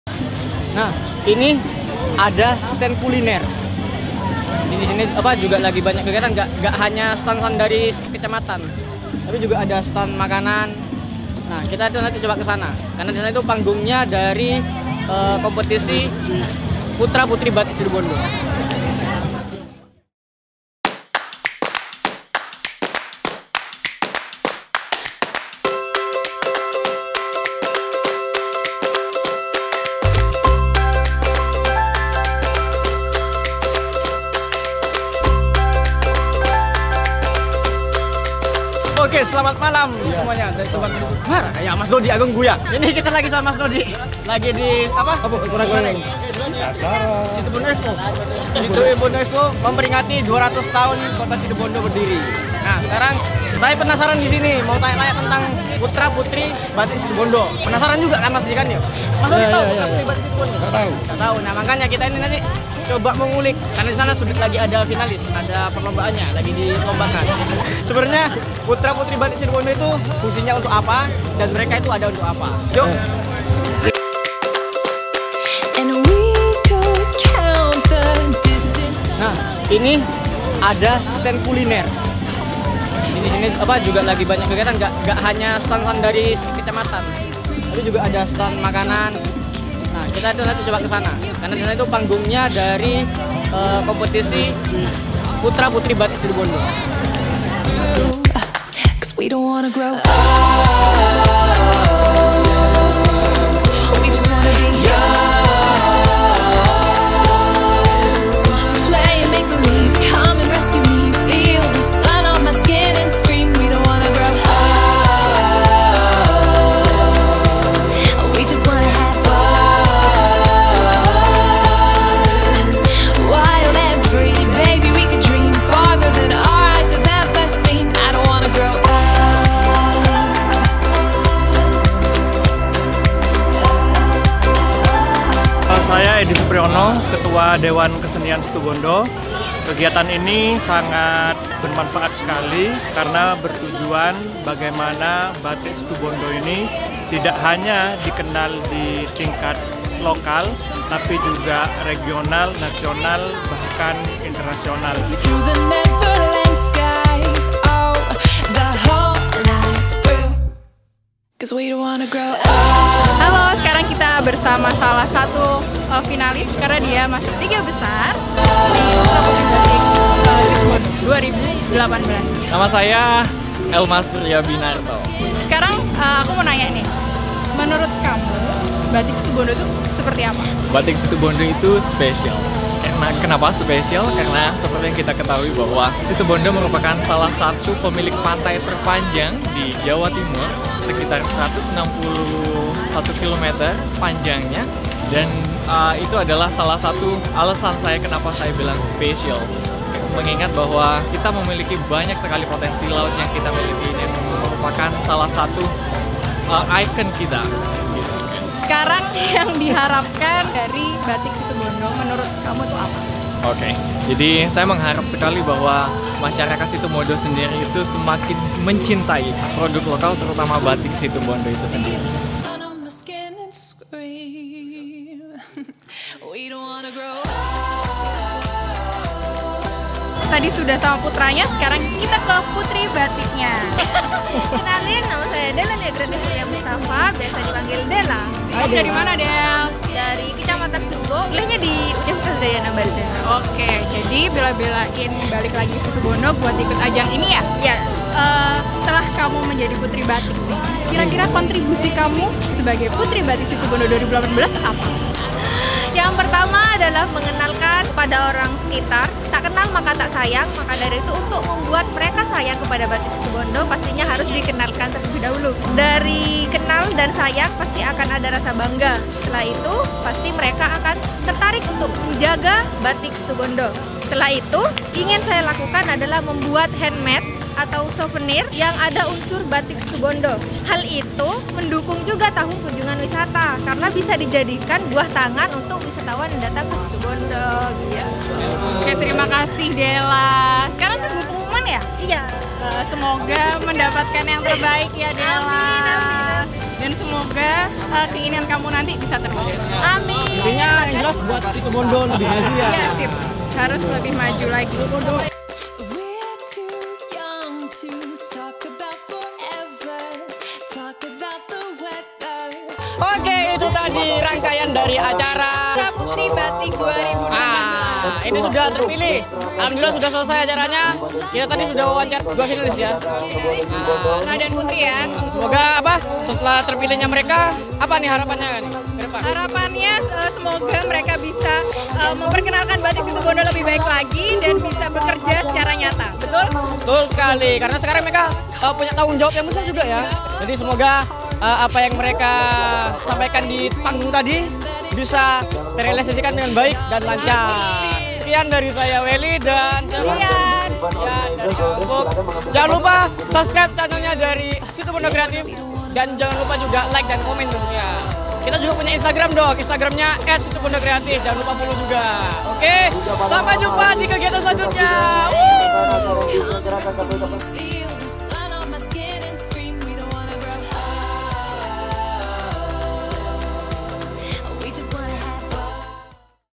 BATIK SITUBONDO 2018 | Situbondo Kreatif
BATIK SITUBONDO 2018
Batik Situbondo adalah Batik asli yang berasal dari kabupaten Situbondo. Motif batik ini kebanyakan bermotif kerang karena utara Kabupaten Situbondo ini terdapat pesisir. Sejarah batik telah sejak lama, banyak versi tentang keberadaan batik di Situbondo zaman dahulu
Situbondo Kreatif